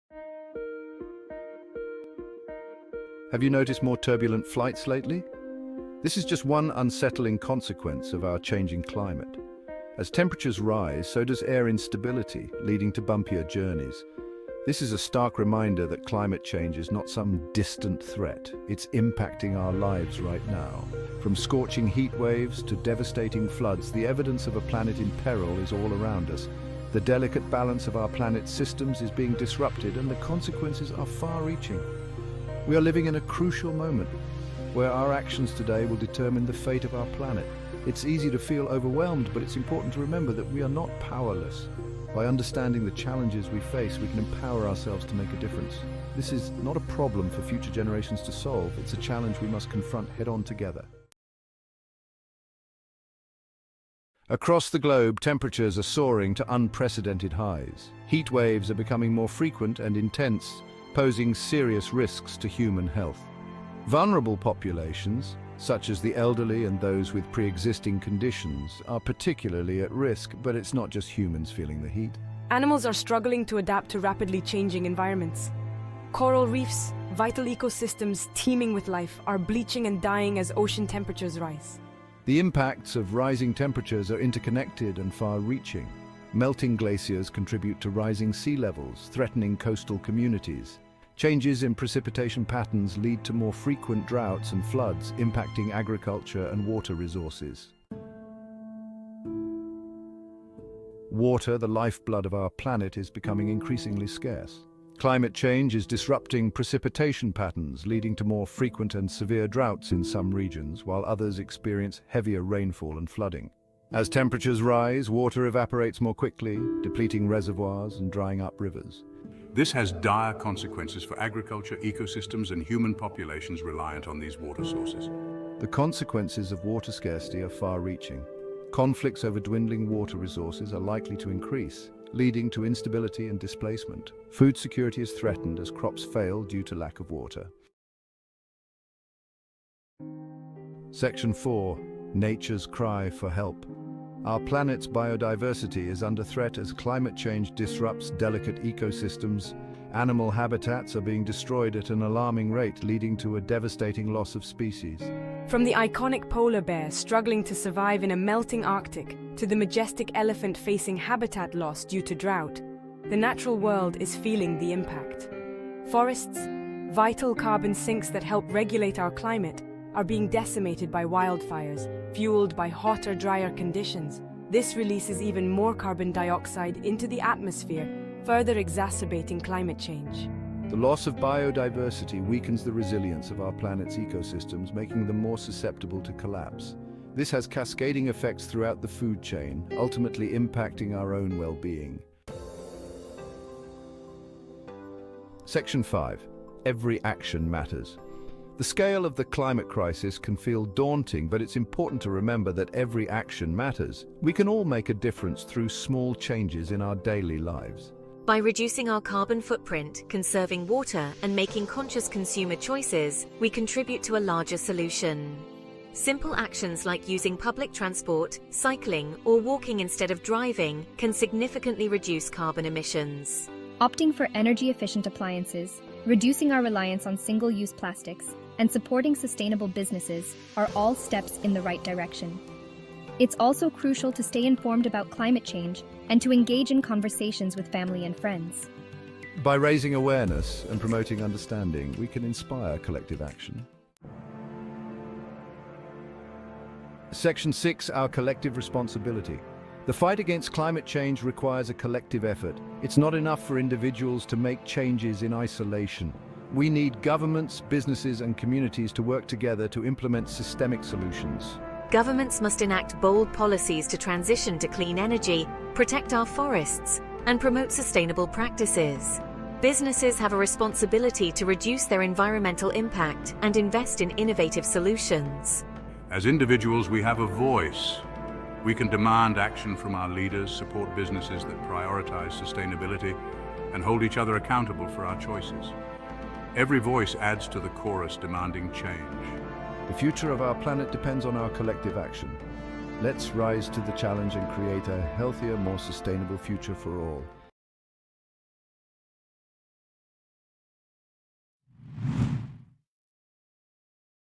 Climate Change - Our Planet in Peril | TALRadio English
Discover practical and impactful ways we can all contribute to the fight against climate change. This short documentary highlights actionable strategies and everyday habits that can make a significant difference. From reducing carbon footprints and embracing renewable energy to supporting sustainable practices and advocating for policy changes, learn how individuals and communities can work together to create a healthier planet. Join us in taking steps towards a sustainable future.